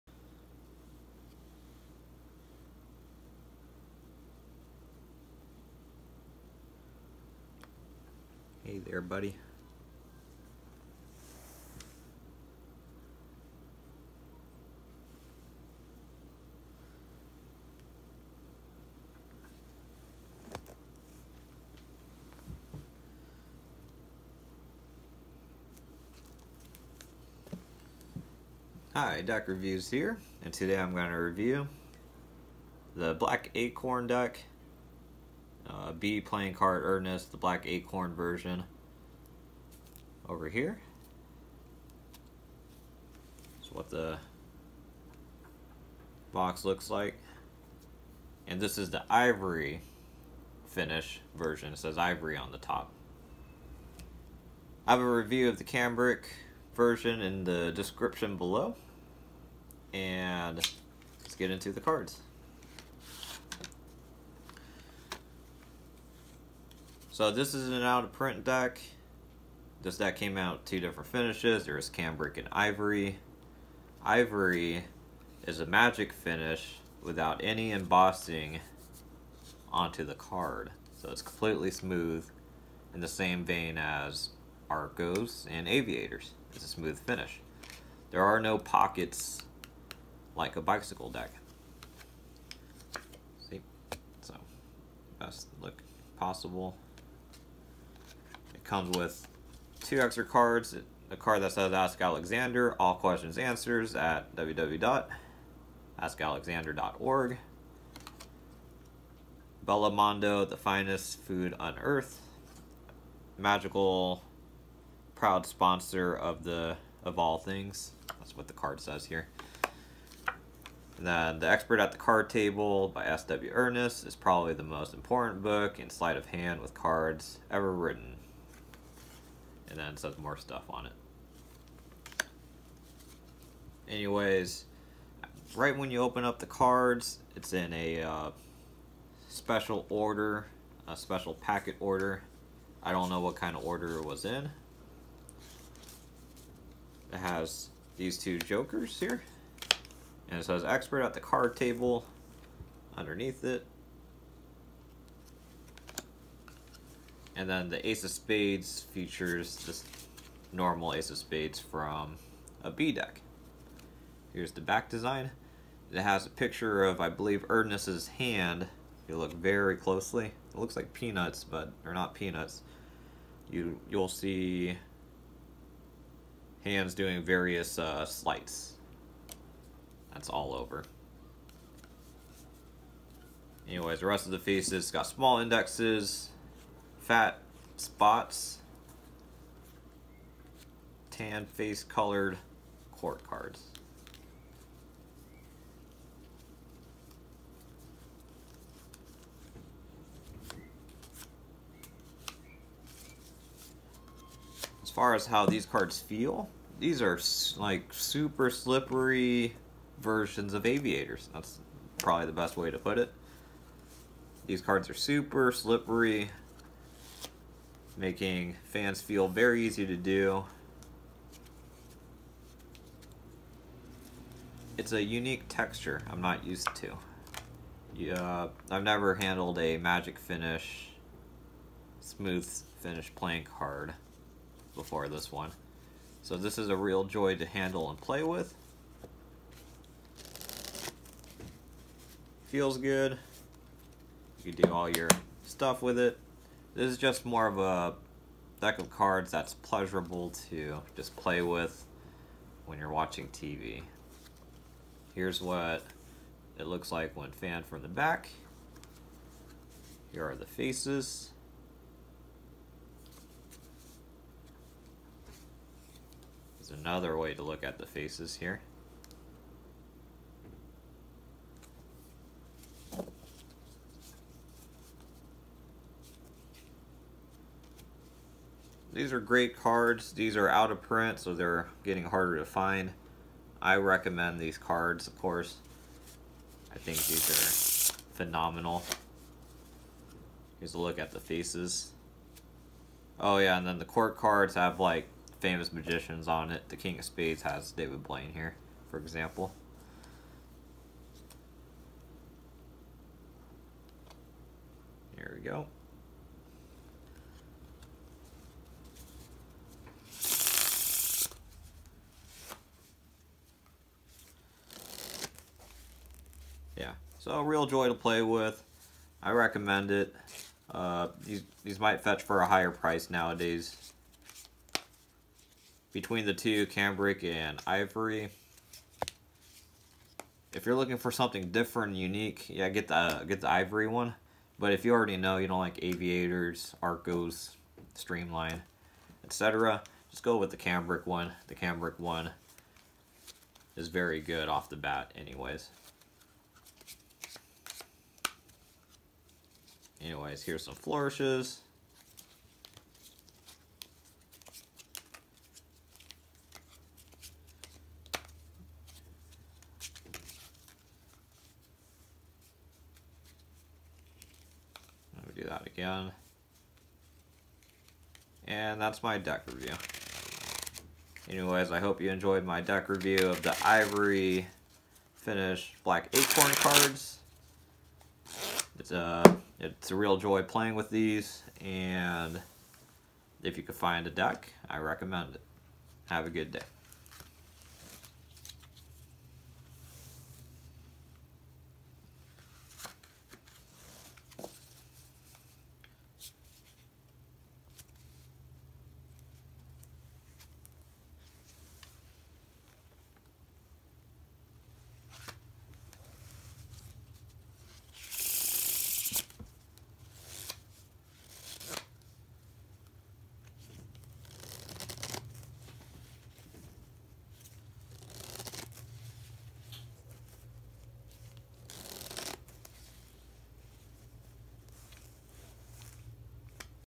Black Acorn Deck Review (Ivory Finish)
Deck review of the black acorn deck with Ivory finish.

Here’s the video on the Cambric version below

Learn the double fan in the link below here